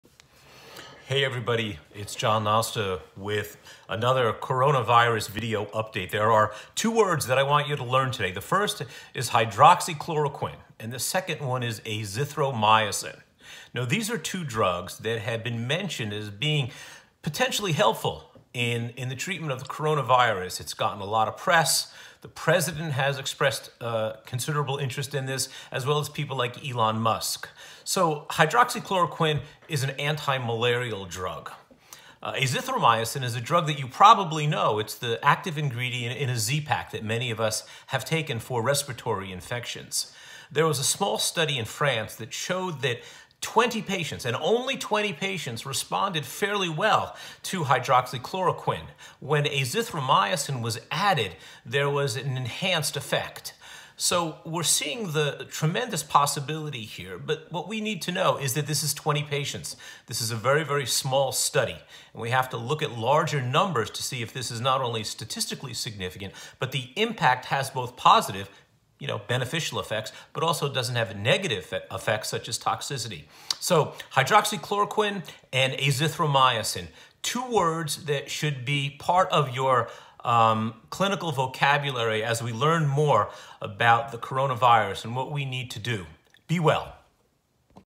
A quick review on hydroxychloroquine and azithromycin for the Corona virus
Here’s a brief overview of these two drugs that have shown potential use in treating the Corona virus.